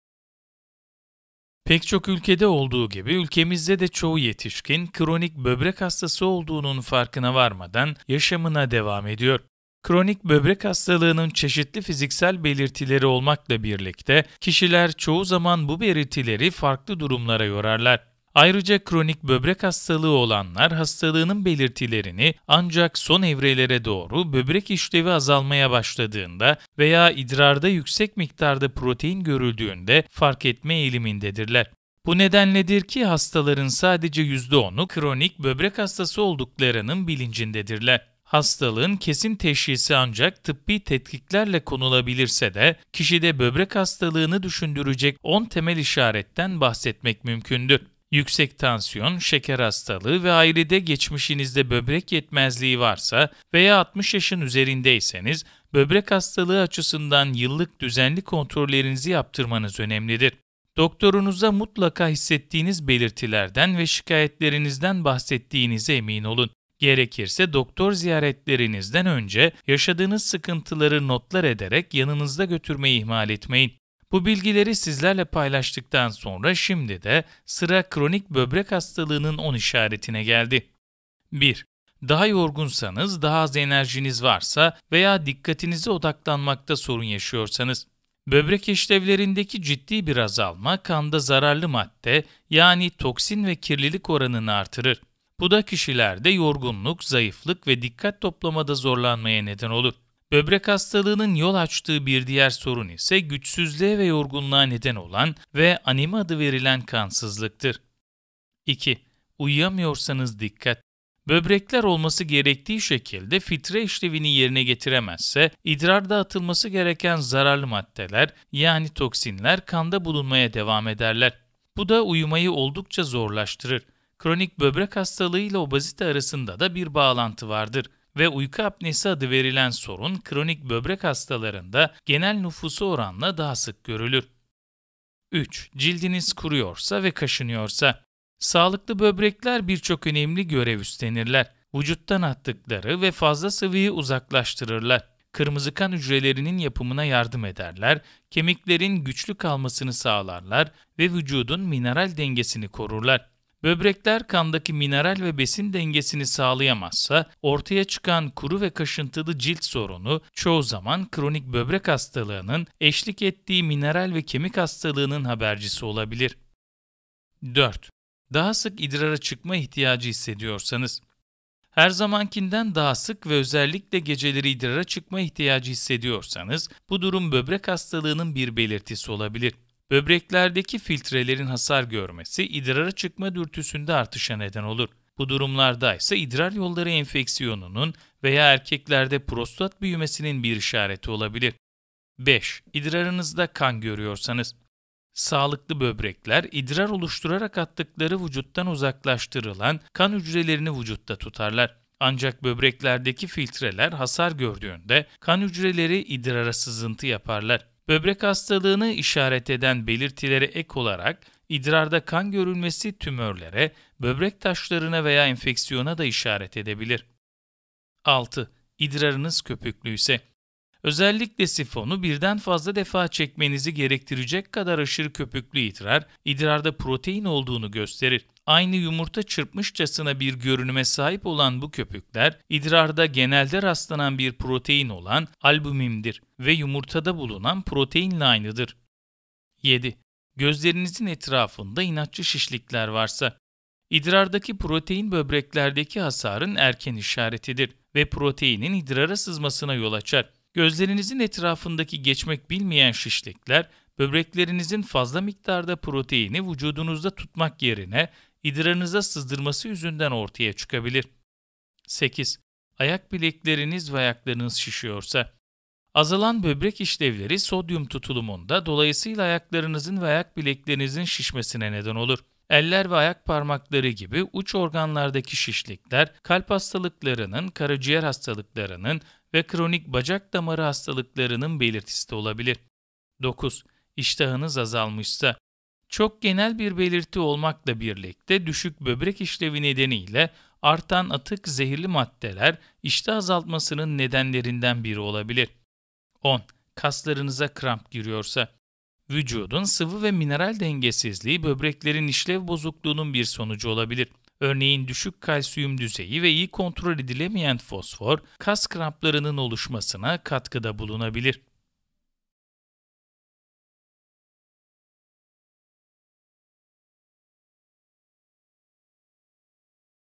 Kronik Böbrek Hastalığının 10 İşareti! Kronik böbrek hastalığı nedir?
Pek çok ülkede olduğu gibi ülkemizde de çoğu yetişkin, kronik böbrek hastası olduğunun farkına varmadan yaşamına devam ediyor. Kronik böbrek hastalığının çeşitli fiziksel belirtileri olmakla birlikte, kişiler çoğu zaman bu belirtileri farklı durumlara yorarlar. Ayrıca kronik böbrek hastalığı olanlar, hastalığın belirtilerini ancak son evrelere doğru, böbrek işlevleri azalmaya başladığında veya idrarda yüksek miktarda protein görüldüğünde fark etmeye eğilimlidirler. Bu nedenledir ki, hastaların sadece % 10’u kronik böbrek hastası olduklarının bilincindedirler. Peki kronik böbrek hastalığı nedir? Kronik Böbrek Hastalığının 10 İşareti nedir?

00:00 Kronik böbrek hastalığı nedir?
01:20 Kronik böbrek hastalığının 10 işareti